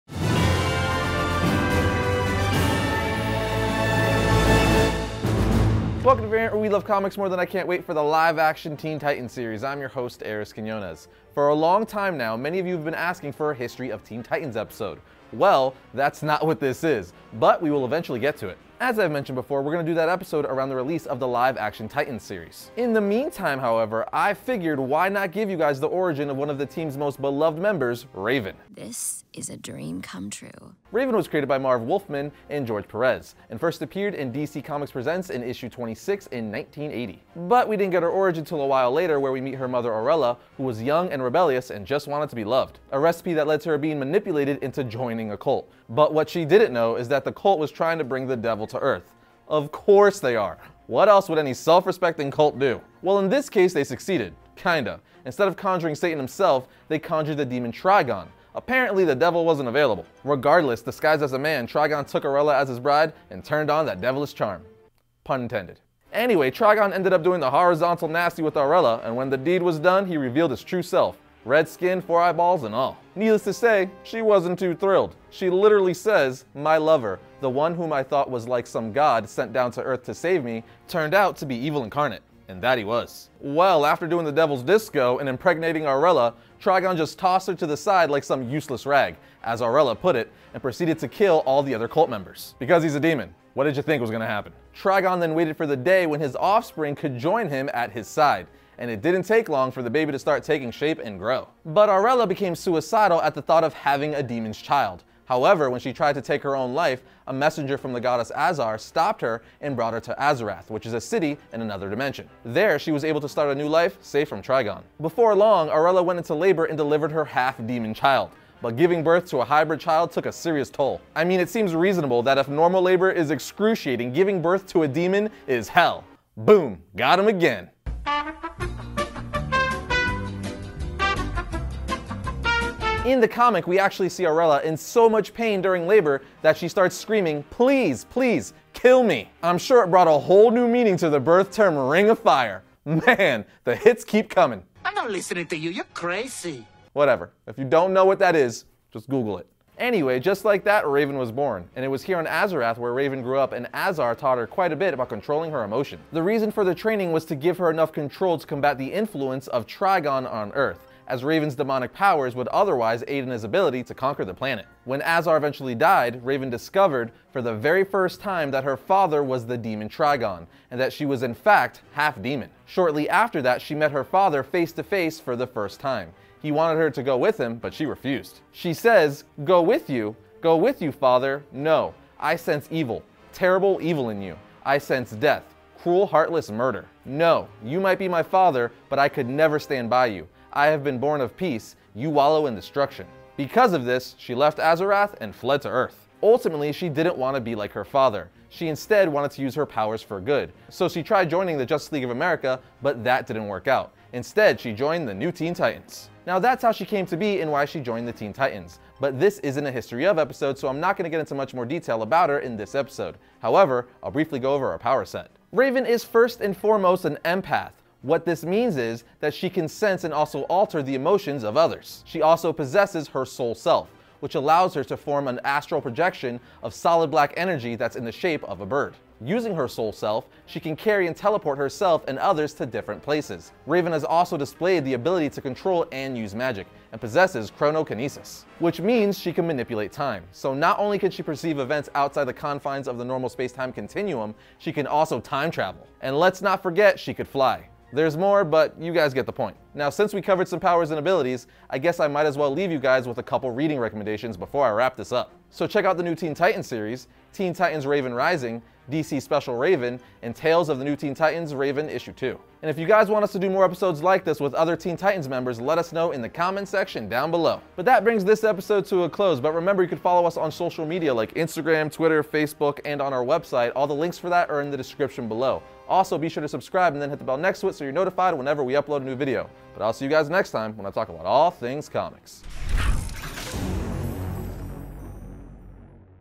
Origin Of Raven (Teen Titans)
Today on Variant, we finally give you guys some Teen Titan goodness by talking about the origin of one of it's most popular members, Raven!